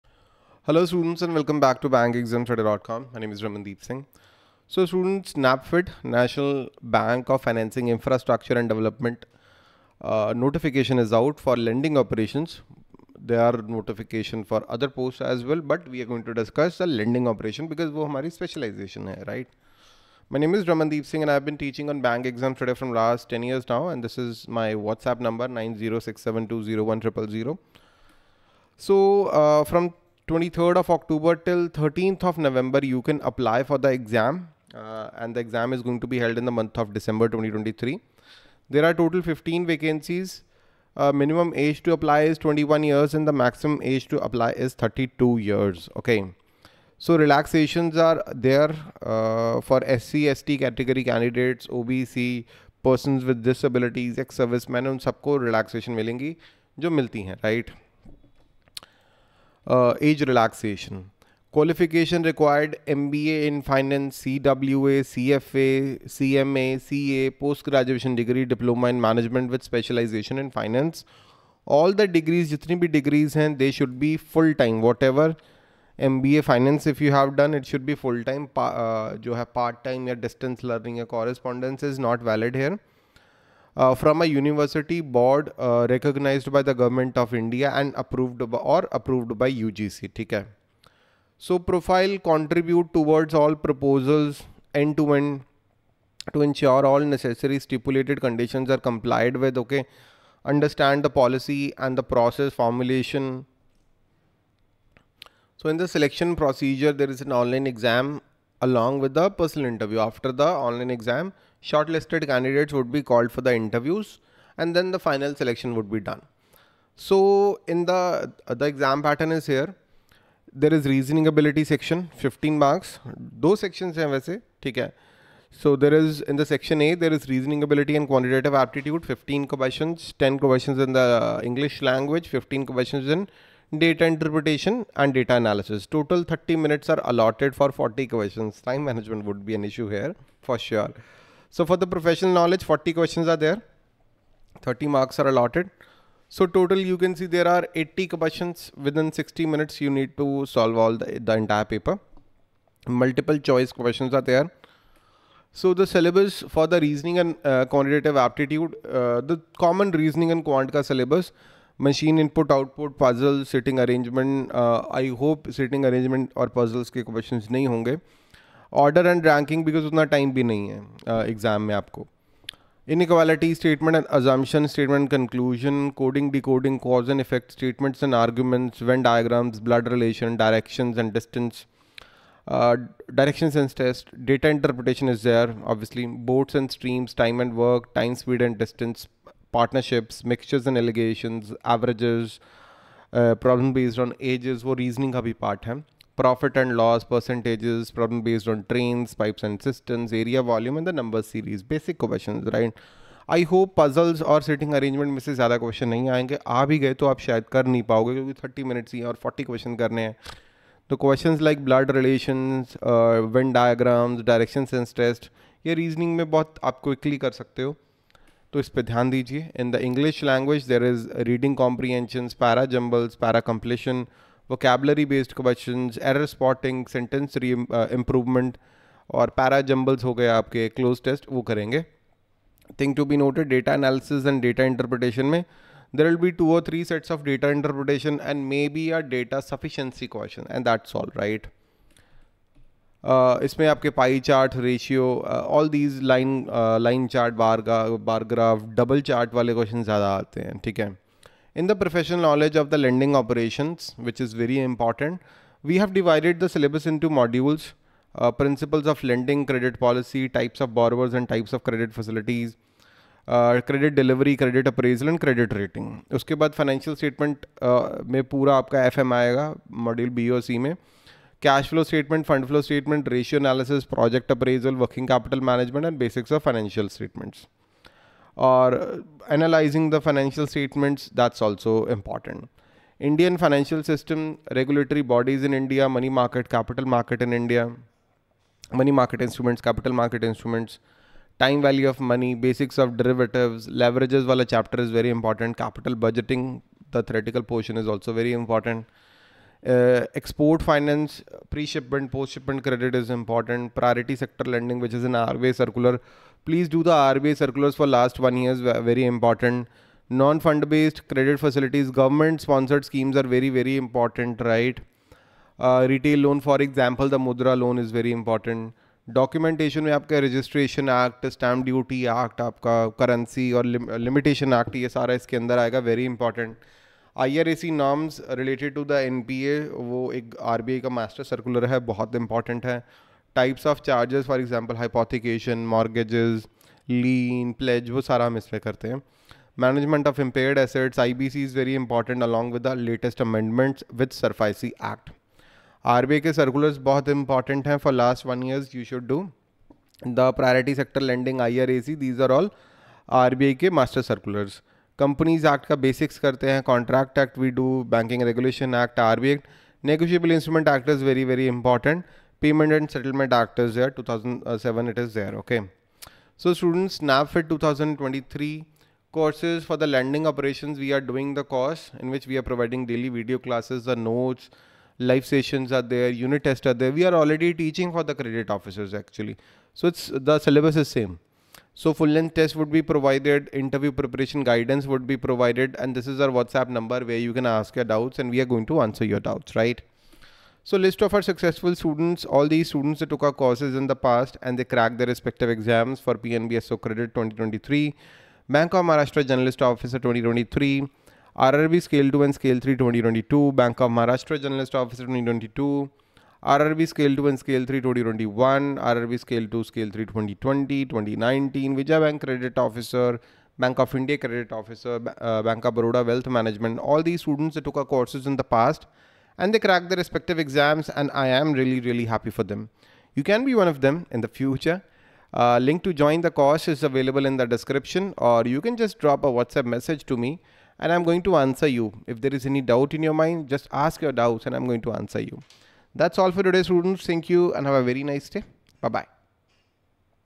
NaBFID Lending Operations Recruitment 2023
NaBFID is one of the principal entities for infrastructure financing in the country that was set up under the NaBFID Act, 2021. NaBFID has invited online applications from eligible Indian citizens for the regular basis recruitment of 15 Lending Operations posts.

The online application process has started from 24th October 2023 and will end on 13th November 2023. The selection of the candidates will be done on the basis of a computer-based test, which will be conducted in the month of November or December 2023. The candidates who qualify the test will be called for an interview. The candidates who are interested in applying for the NaBFID Officer Recruitment 2023 must have the required educational qualification, age limit, and experience as per the notification.